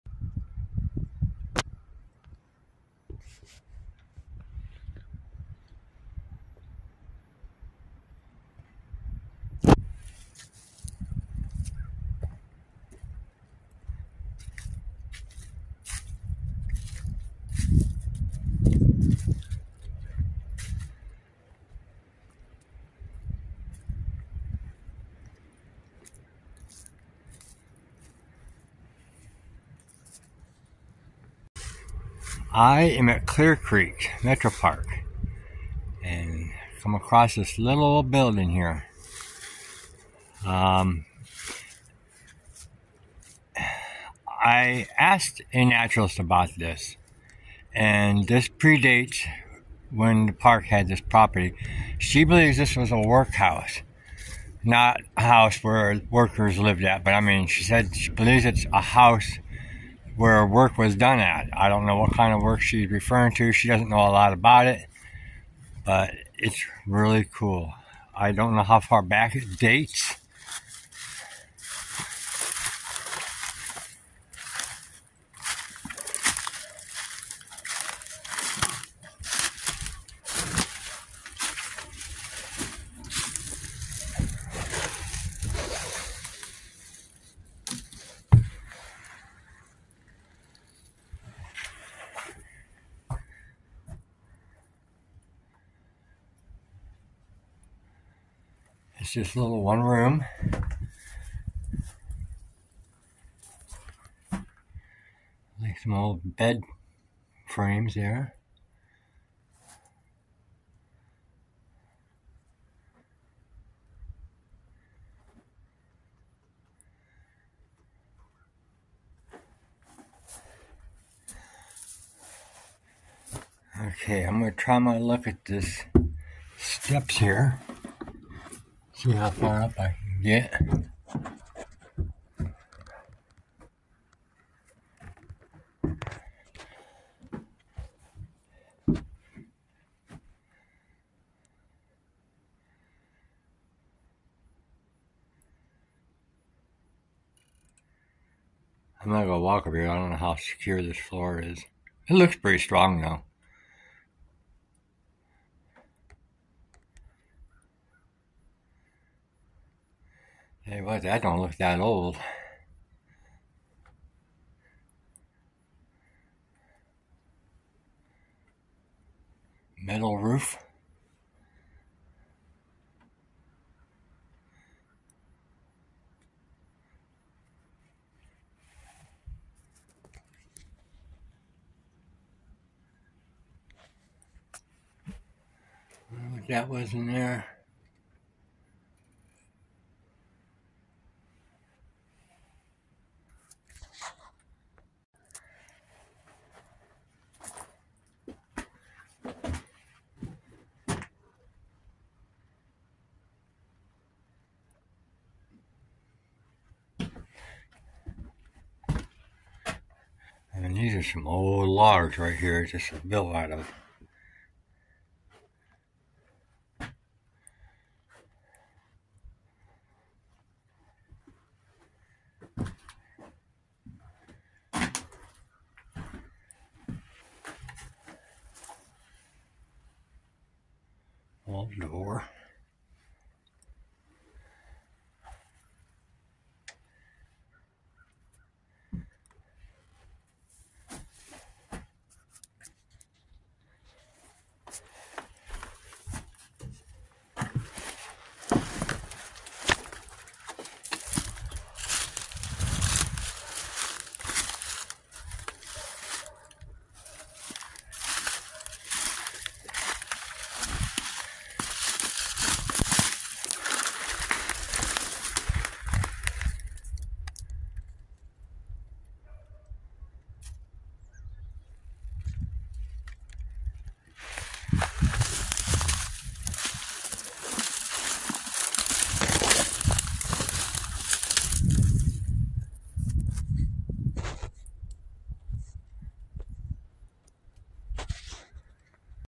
Rustic House at Clear Creek Metro Park
This old rustic house is at Clear Creek Metro Park and just one of many structures still standing in the park that date back to before the park was established